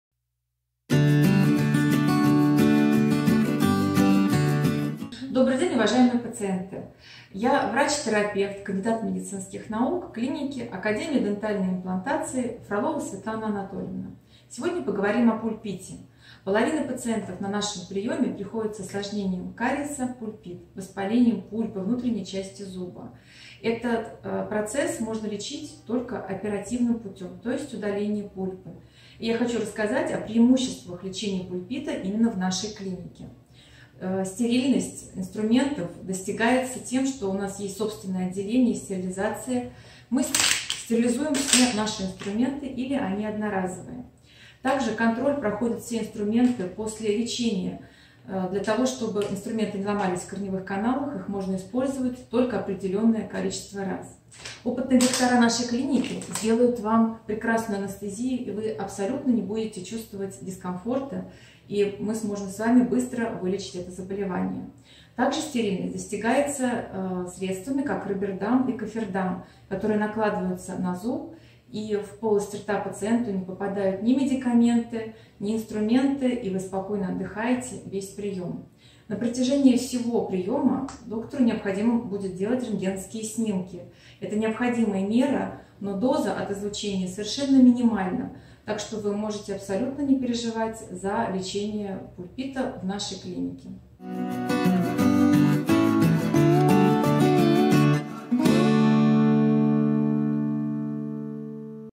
Лечение пульпита_Мегастом
Кандидат медицинских наук, ведущий стоматолог-терапевт клиники "Академия Дентальной Имплантации" Фролова Светлана Анатольевна поделилась информацией о том, как лечить пульпит и каковы преимущества лечения пульпита в клиниках "Мегастом"